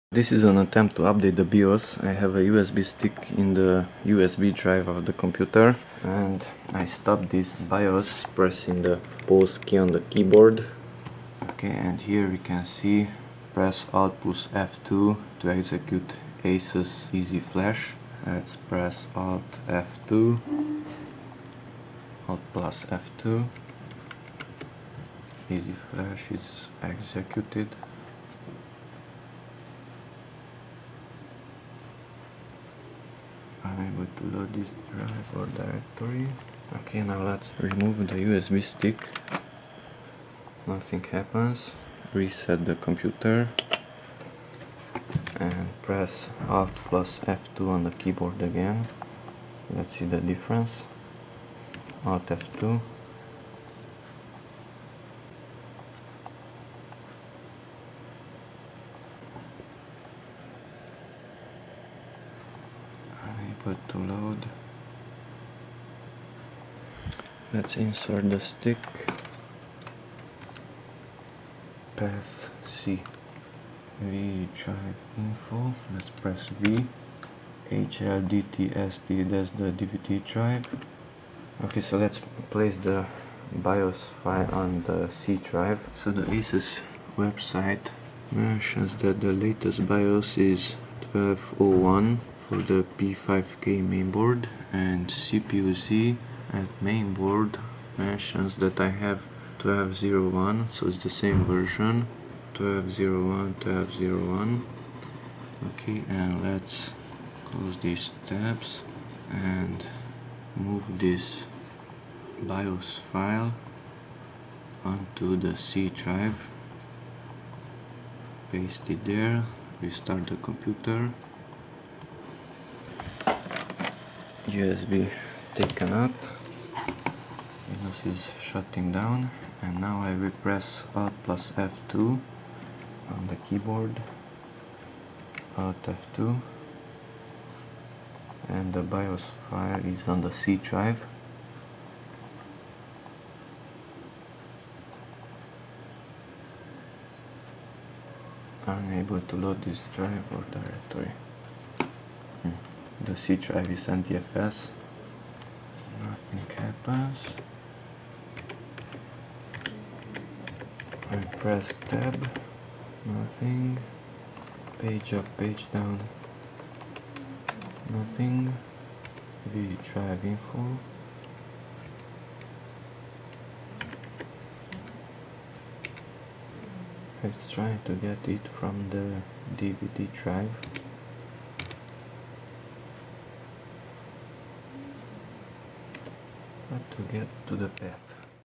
BIOS update attempt on ASUS P5K motherboard
The update was unsuccessful. There were no updates available at that time on the ASUS website.
The BIOS update file was expected from the CD/DVD drive.
The CD/DVD drive was recognized as drive C.

Under Windows, CPU-Z was used to determine the motherboard's make and model.